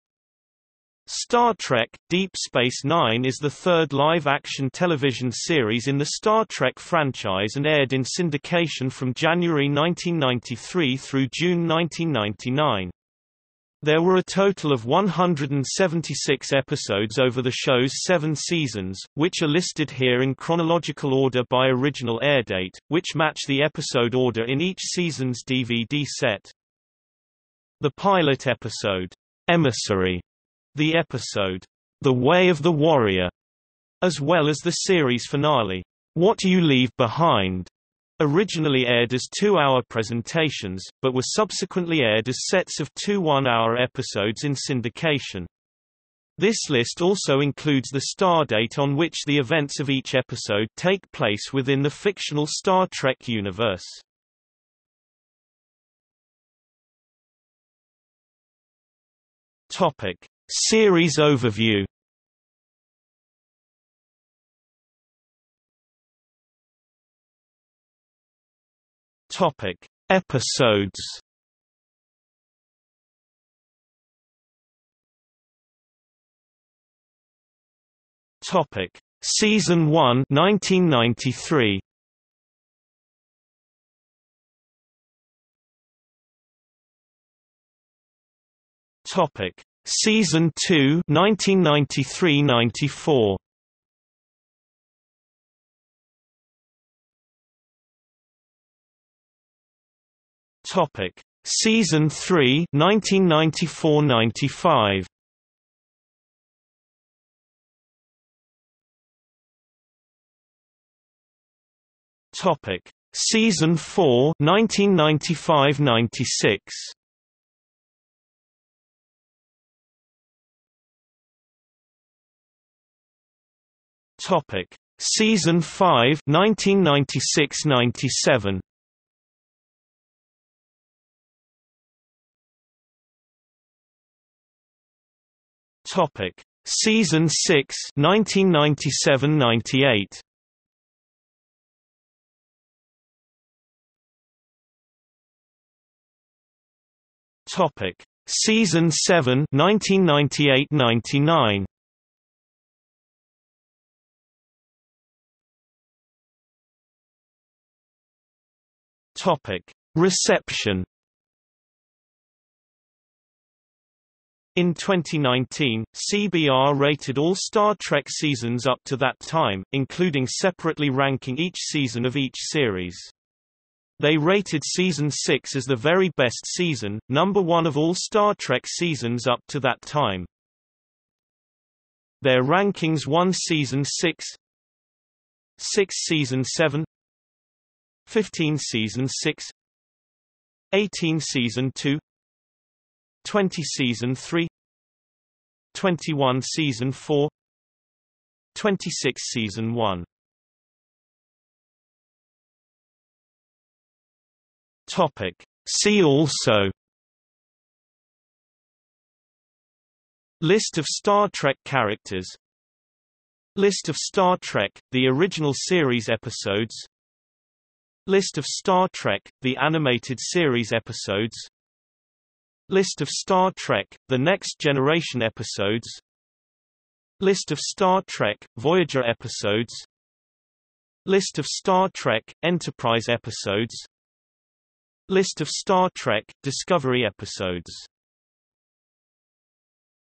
Star Trek: Deep Space Nine (season 2) | Wikipedia audio article
00:00:56 1 Series overview
00:01:05 2 Episodes
00:01:15 2.1 Season 1 (1993)
00:01:25 2.2 Season 2 (1993–94)
00:01:37 2.3 Season 3 (1994–95)
00:01:48 2.4 Season 4 (1995–96)
00:02:00 2.5 Season 5 (1996–97)
00:02:12 2.6 Season 6 (1997–98)
00:02:24 2.7 Season 7 (1998–99)
00:02:35 3 Reception
00:03:21 4 See also

- Socrates

SUMMARY
Star Trek: Deep Space Nine is the third live-action television series in the Star Trek franchise and aired in syndication from January 1993 through June 1999. There were a total of 176 episodes over the show's seven seasons, which are listed here in chronological order by original airdate, which match the episode order in each season's DVD set.
The pilot episode, "Emissary", the episode "The Way of the Warrior", as well as the series finale, "What You Leave Behind", originally aired as two-hour presentations, but were subsequently aired as sets of two one-hour episodes in syndication. This list also includes the stardate on which the events of each episode take place within the fictional Star Trek universe.